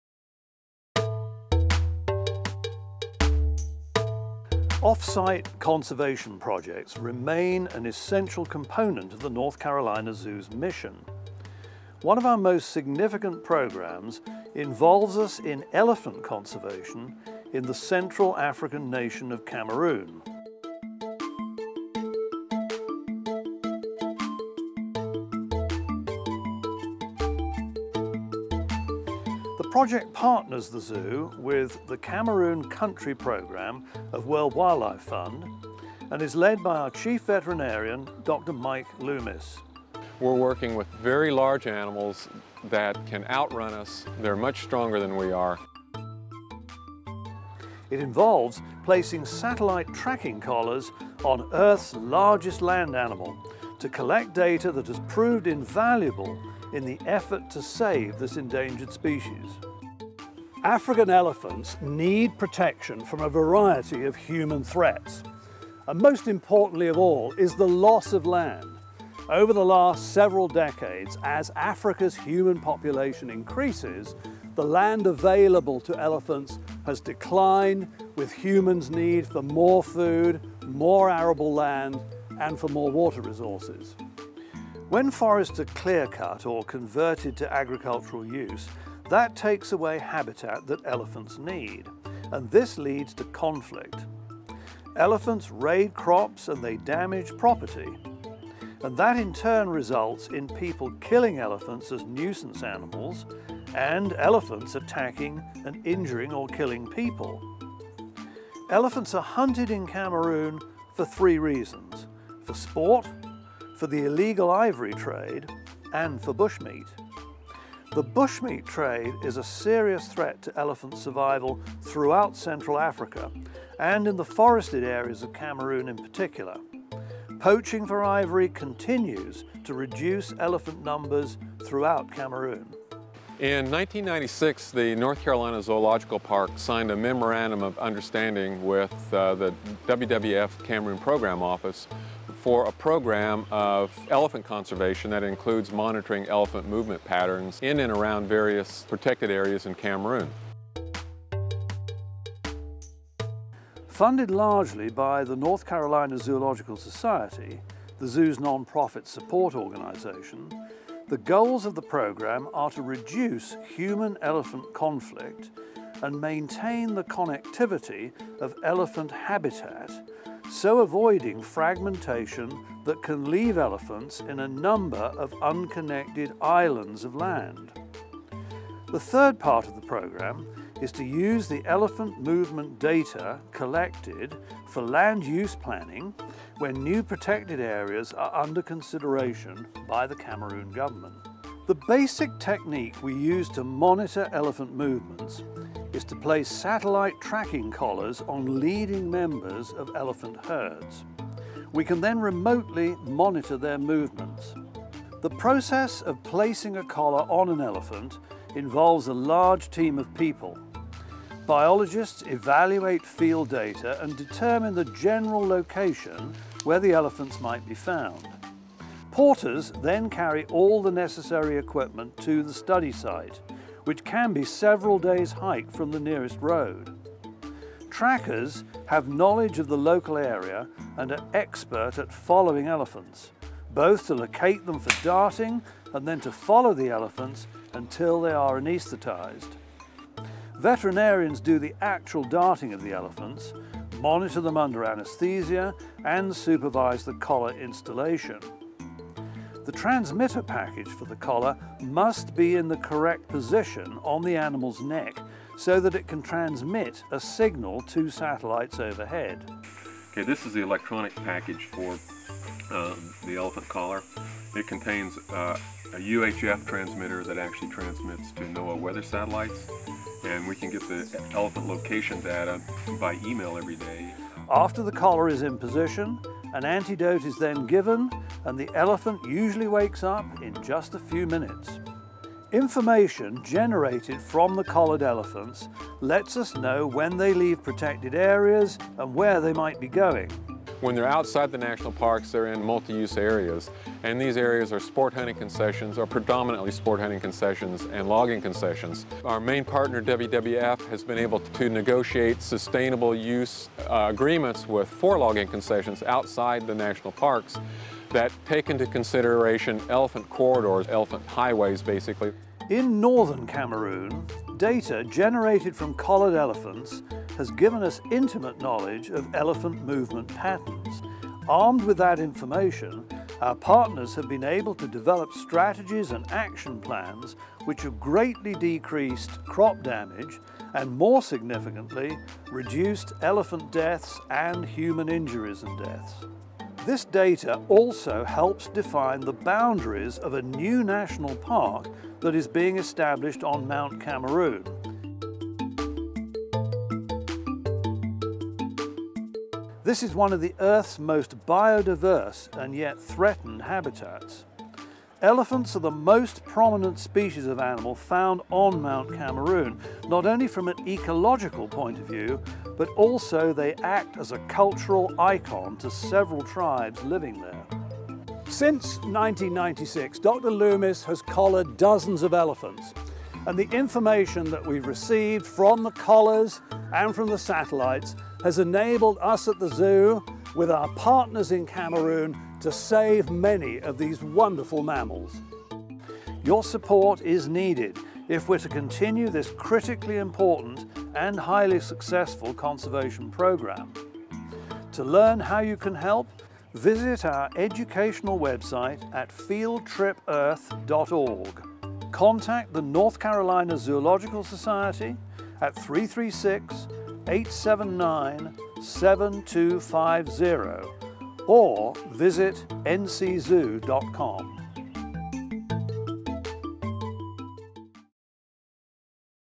The Cameroon Elephant Project: Saving Earth's Largest Land Mammal
The North Carolina Zoo's commitment to wildlilfe extends far beyond its gates. One of the Zoo's most important conservation research projects operates in the western African nation of Cameroon. There, Zoo Chief Veterinarian Dr. Michael Loomis works with several NGO and government partners to study elephants and elephant habitat.

The Zoo joined forces with the World Wildlife Fund (WWF) in 1996 to study elephants in northern Cameroon. The project has grown since then to include studies in the southeast forests and on Mount Cameroon as well. Dr. Loomis and his colleagues have deployed satellite tracking collars on dozens of elephants. The data collected is then analyzed and visualized in order to better understand elephant movement patterns, habitat usage and other factors. With these data in hand, elephant conservationists are working to limit human/elephant conflicts, to maintain the connectivity of existing elephant habitat, and to improve land-use planning decisions in several areas of Cameroon.